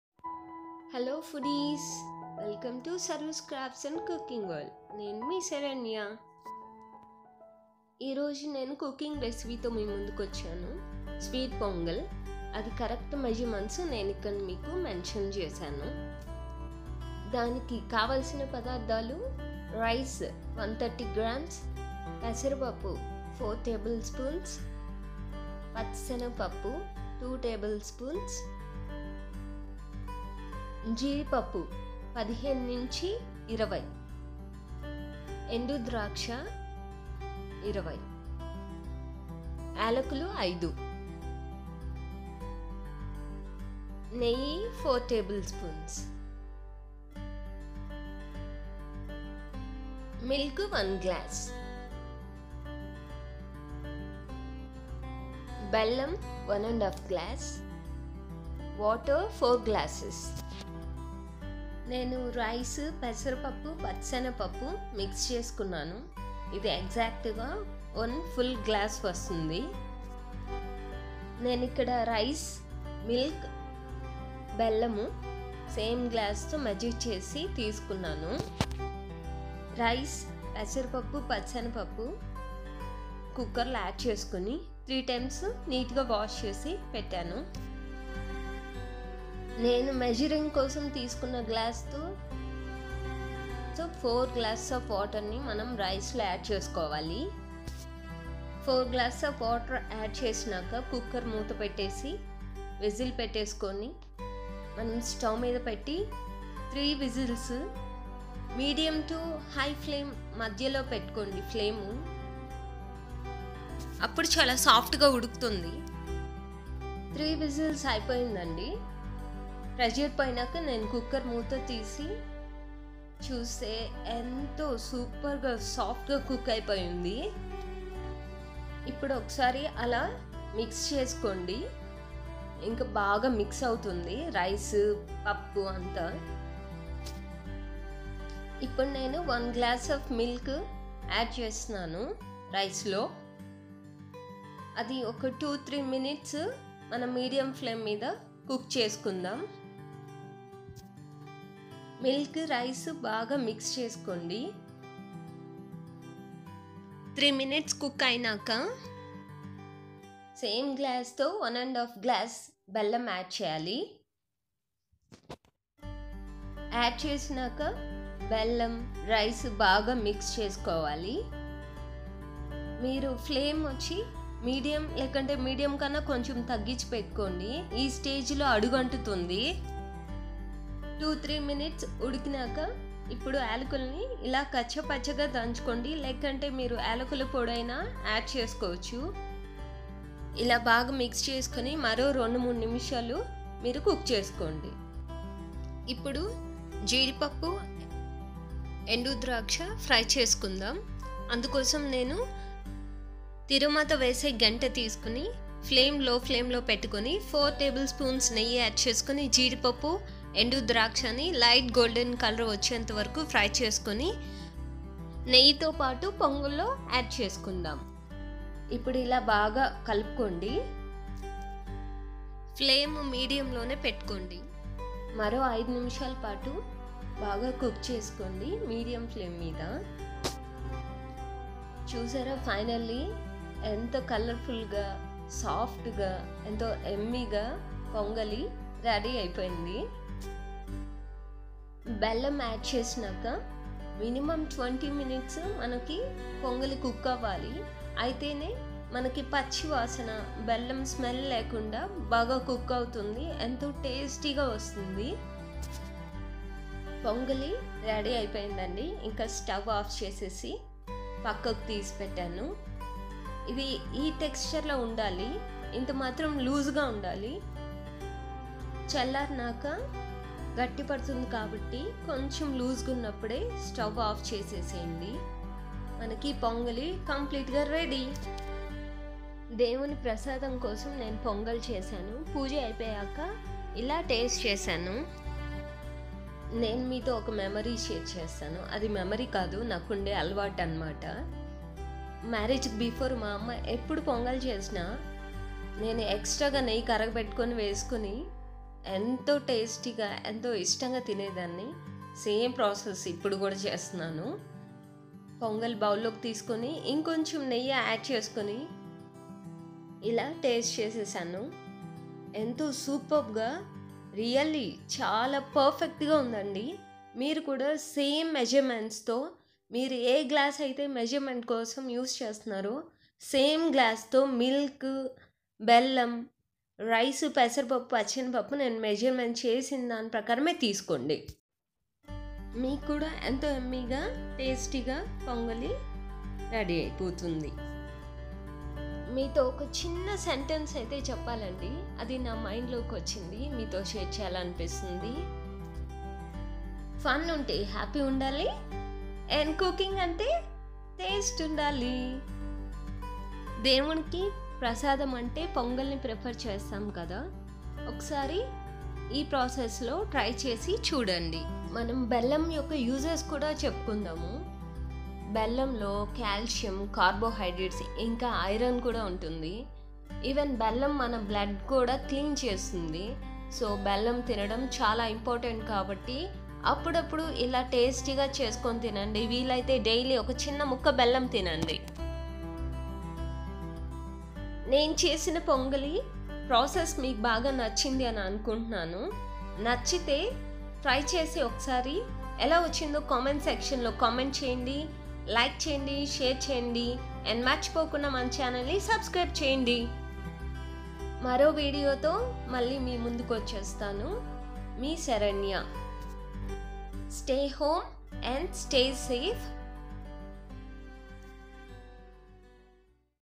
స్వీట్ పొంగలి, పరమాన్నం.. ఎంతో ప్రీతీకరంగా దేవునికీ నైవేద్యంగా పెట్టె పొంగలి...సూపర్ గా 👌👌👌
హాయ్ ఫ్రెండ్స్.

ఈరోజు నేను నా ఫస్ట్ కుకింగ్ రేసీపీ తో మీ ముందుకు వచ్చాను. దేవునికీ నైవేద్యం గా పెట్టె స్వీట్ పొంగలి నేను ఎప్పుడు ఈ ప్రాసెస్ లో చేస్తాను చాలా రుచికరంగా వస్తుంది.

మీరు కూడా ఈ ప్రాసెస్ లో ట్రై చేస్తారని ఆశిస్తున్నాను... నా వీడియో మీకు నచ్చినట్లు అయితే... లైక్ చేయండి, షేర్ చేయండి... ప్లీజ్ సబ్స్క్రయిబ్ చేయండి.

ప్లీజ్ సపోర్ట్ చేయండి. థాంక్యూ.